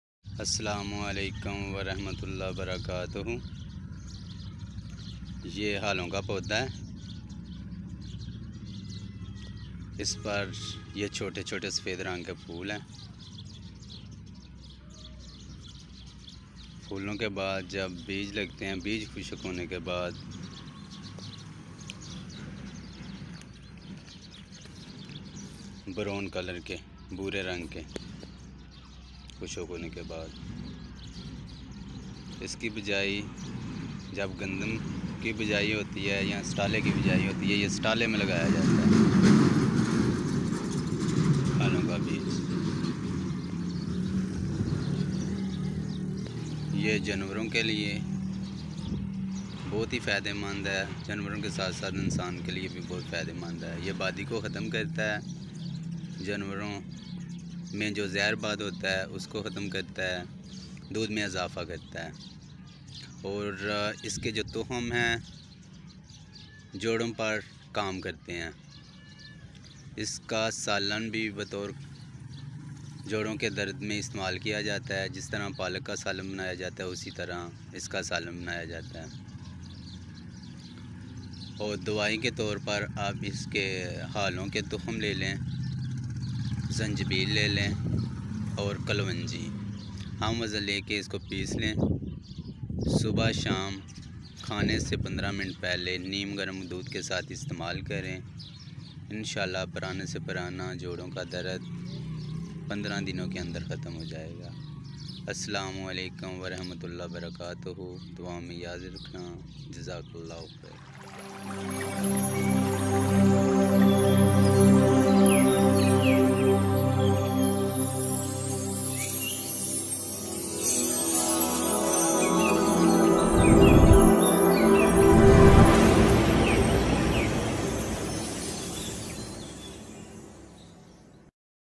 Lepidium sativum،garden gress(ہالوں)se jointsجوڑوںka dard khatam,baadiبادی k lye mufeed Hakeem Azad
kisanorqanoon Lepidium sativum،garden gress(ہالوں)se jointsجوڑوںka dard khatam,baadiبادی k lye mufeed he Hakeem Azad